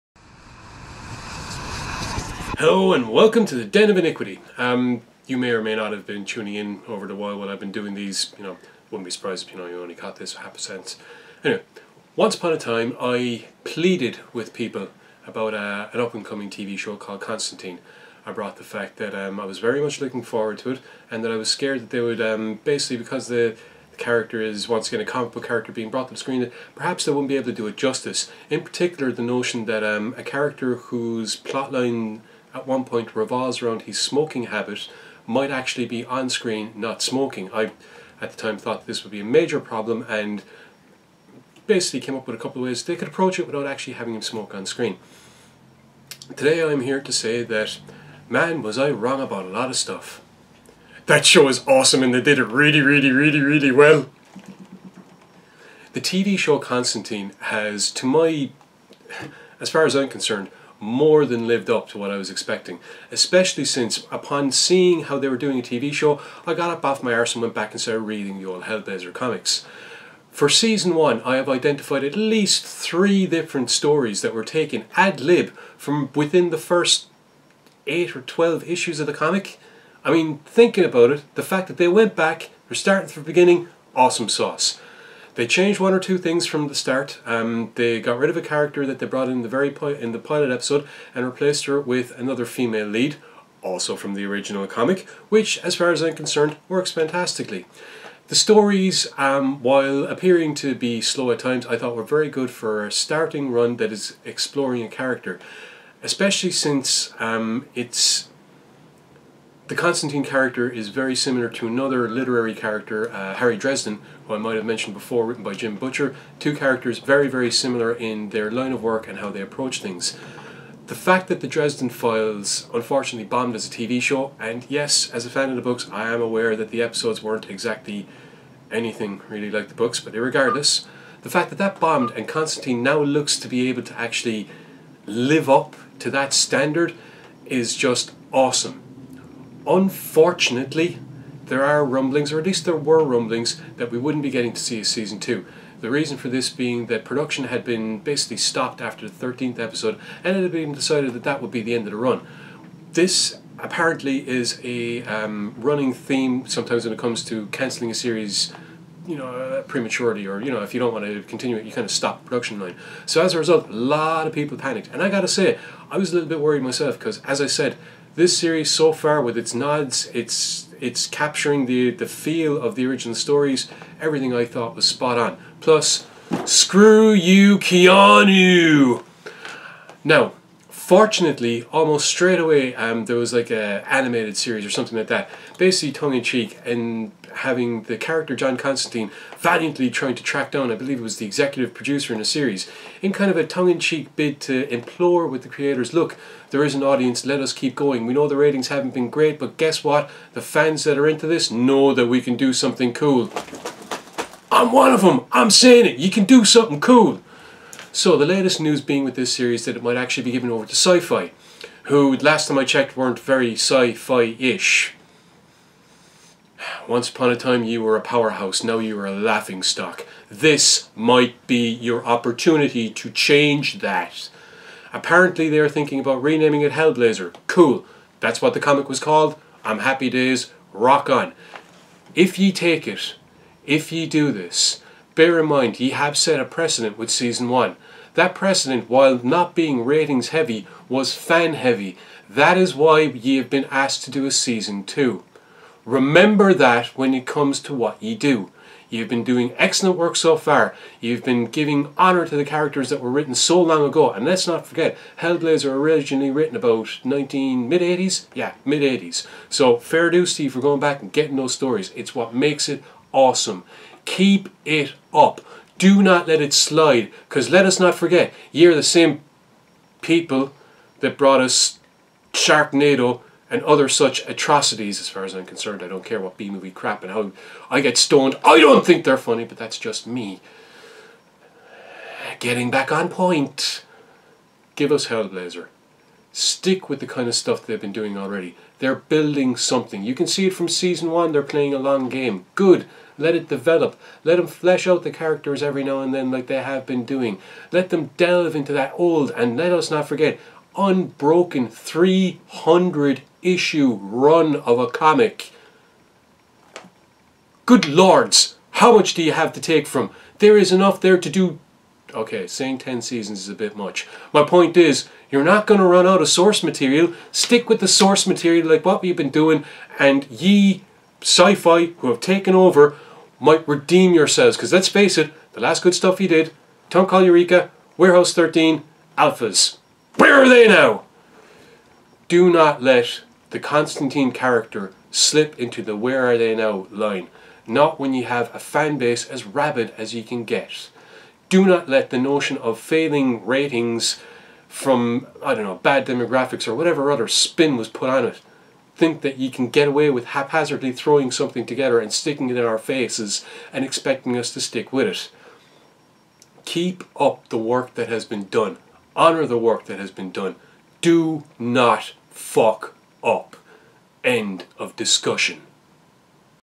Den of Iniquity: Hellblazer & Constantine From Page to Screen
In this weeks Den on Iniquity Giz Discusses the Constantine TV show.